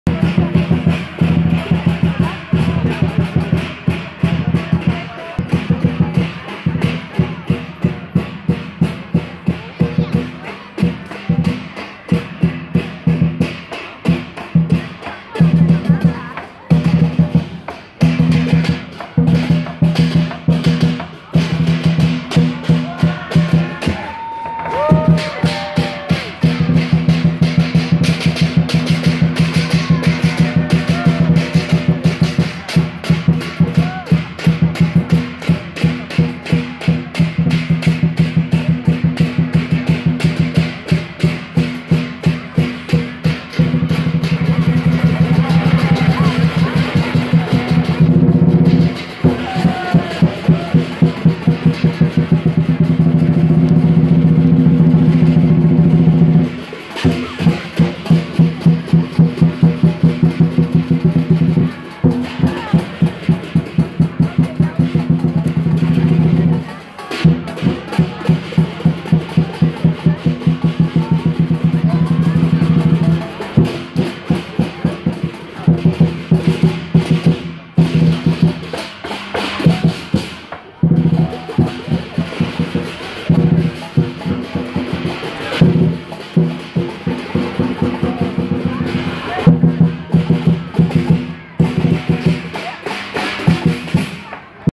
Happy new year , the year of the Rabbit 21 January 2023
We went to Chinatown as we couldn't make it on Sunday  , We had a great day out and enjoy  the dragon dance  happy new year :)